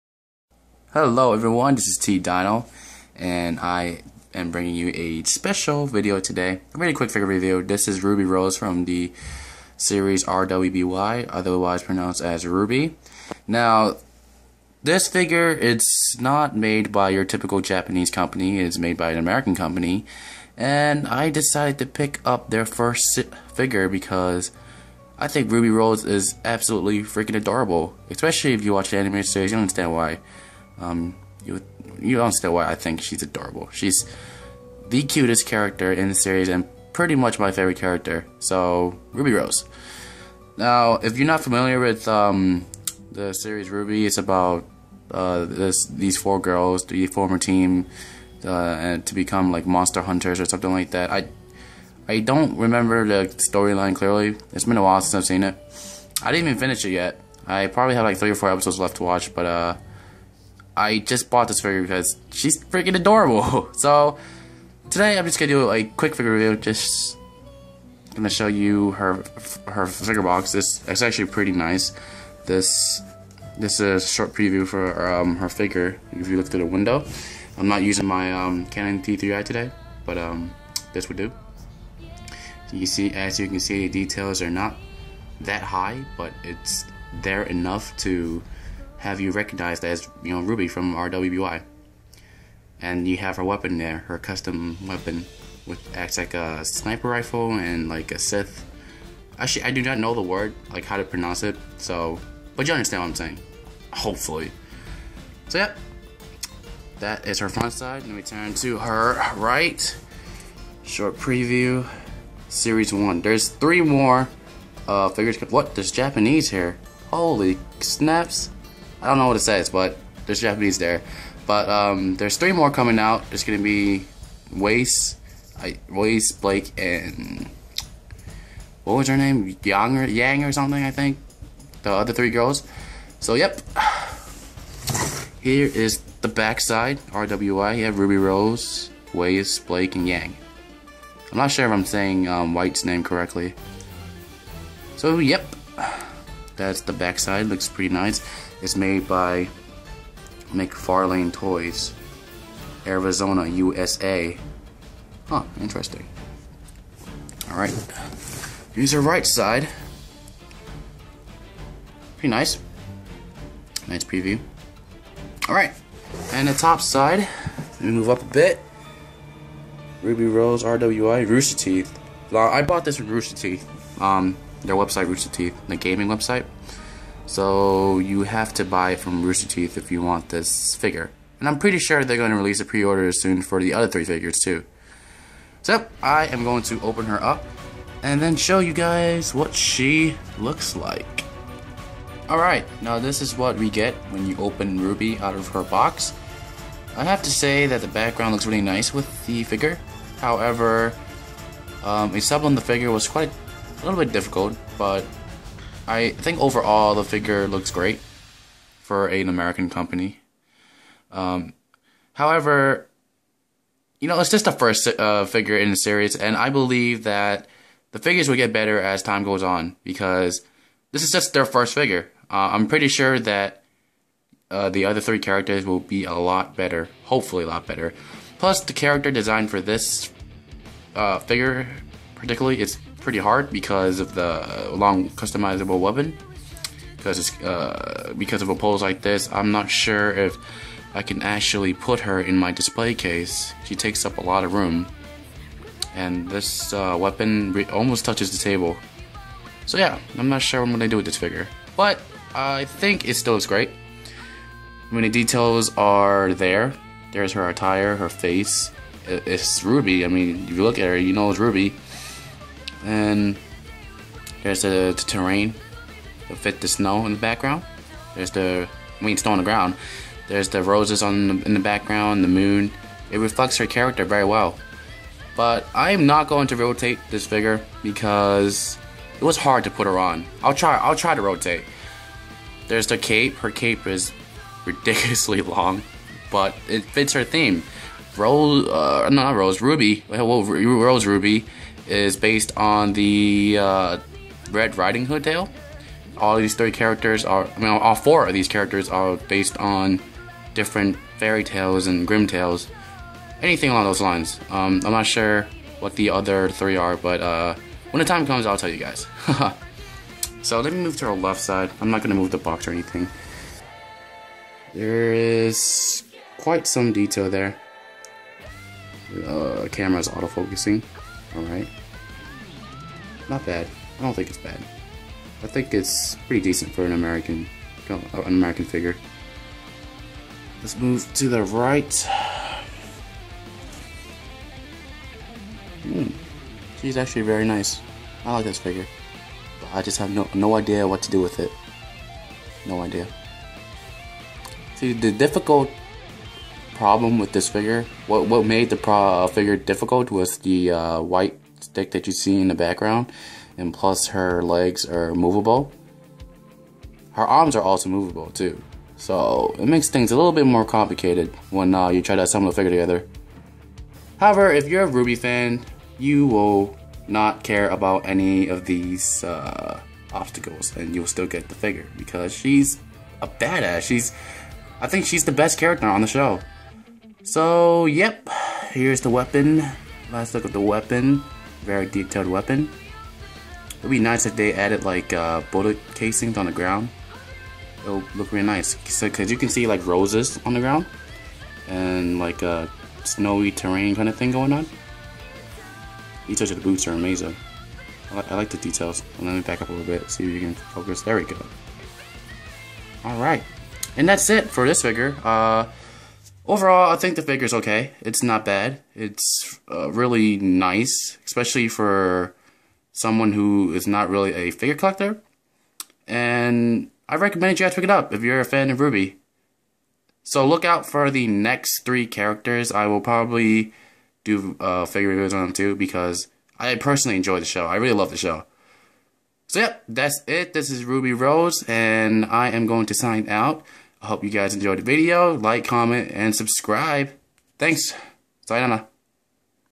RWBY | Ruby Rose - PVC Figure by McFarlane Toys
Check out this nice toy by McFarlane Toys. I LOVE RUBY, she's so adorable!